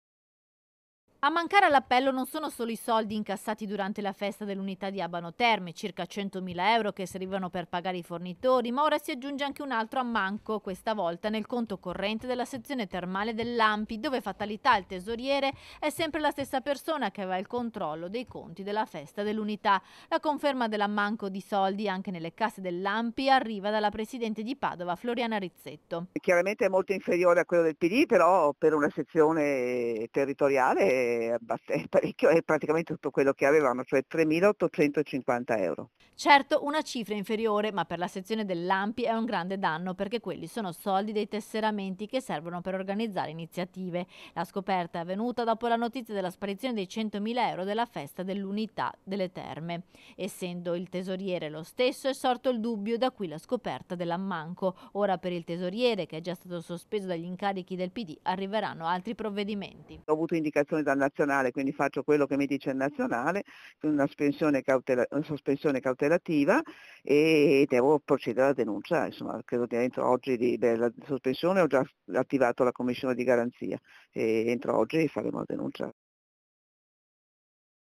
SPARITI I SOLDI DELLA CONTO DELL'ANPI: IL TESORIERE ERA LO STESSO DEL PD DELLE TERME | 01/02/2024
TG Padova Sera 01/02/2024 - ABANO TERME - Spariti i soldi anche dalle casse dell'Anpi delle terme, il tesoriere è lo stesso accusato di aver fatto sparire i soldi della festa dell'Unità || A mancare all'appello non sono solo i soldi incassati durante la festa dell'Unità di Abano Terme, circa 100 mila euro che servivano per pagare i fornitori, ma ora si aggiunge un altro ammanco questa volta nel conto corrente della sezione termale dell'Anpi, dove fatalità il tesoriere è sempre la stessa persona che aveva il controllo dei conti della festa dell'Unità. La conferma dell'ammanco di soldi anche nelle casse dell'Anpi arriva dalla presidente di Padova Floriana Rizzetto.Certo una cifra inferiore, ma per la sezione dell'Anpi è un grande danno perchè quelli sono soldi dei tesseramenti che servono per organizzare iniziative. La scoperta è avvenuta dopo la notizia della sparizione dei 100 mila euro della festa dell'Unità. Essendo il tesoriere lo stesso, è sorto il dubbio, da qui la scoperta dell'ammanco. Ora per il tesoriere che già stato sospeso dagli incarichi del Pd, arriveranno altri provvedimenti. - Intervistati FLORIANA RIZZETTO - Presidente Anpi Padova (Al telefono) (Servizio di Valentina Visentin) - Segui Telenordest anche sul digitale terrestre! Visita il sito